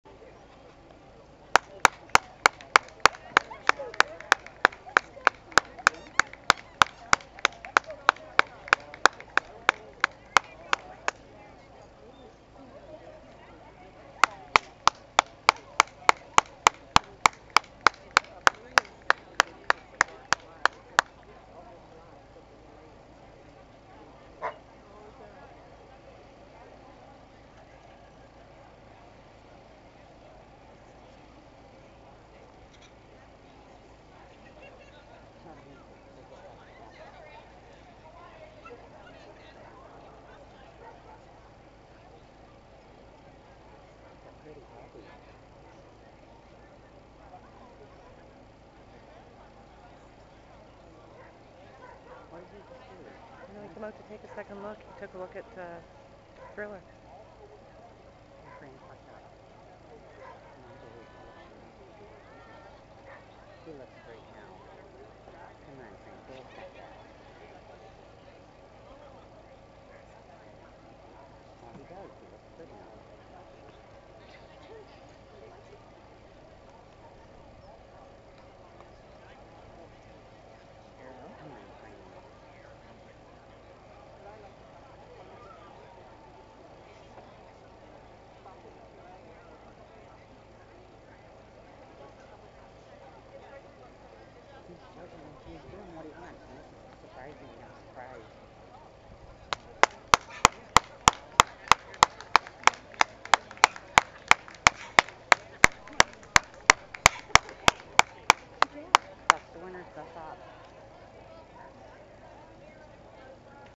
KilbrideBOO.avi
And finally best of Opposites. A 3 point major towards the 10 points need for her Canadian Championship.  We are so happy.